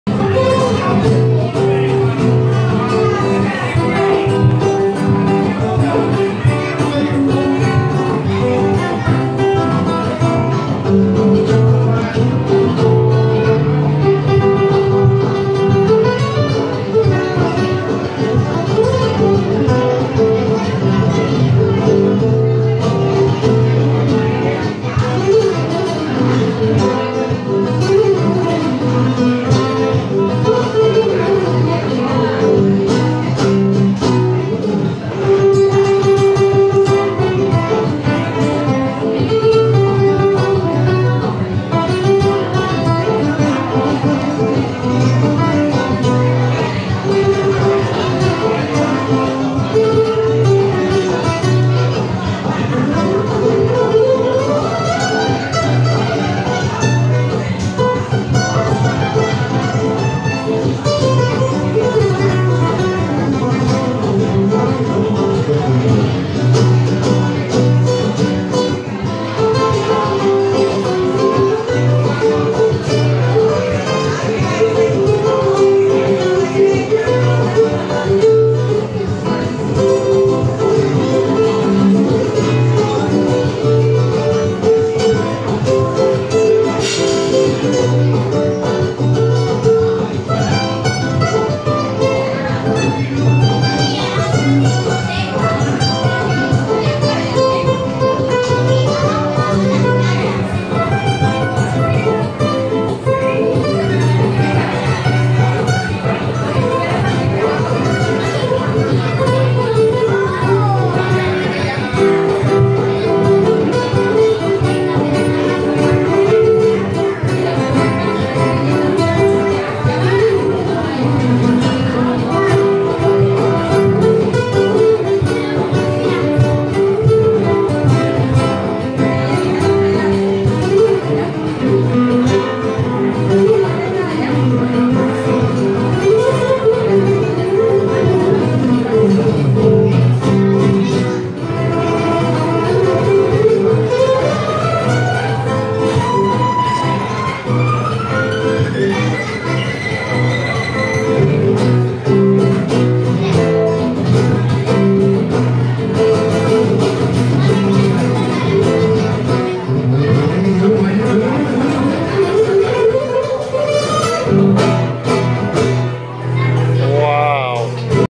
Guitarist at Jimmy's Fish House & Iguana Bar in Clearwater Beach
This was an extended weekend at the beach for my 60th Birthday. Here was a guitarist at Jimmy's Fish House & Iguana Bar in Clearwater Beach. Samsung. 7/21/2019.